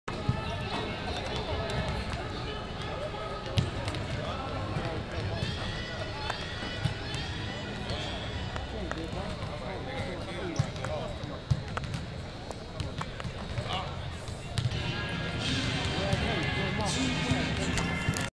Marquis Daniels of The Milwaukee Bucks signing autographs
marquis was cool and signed some autographs while in town with the bucks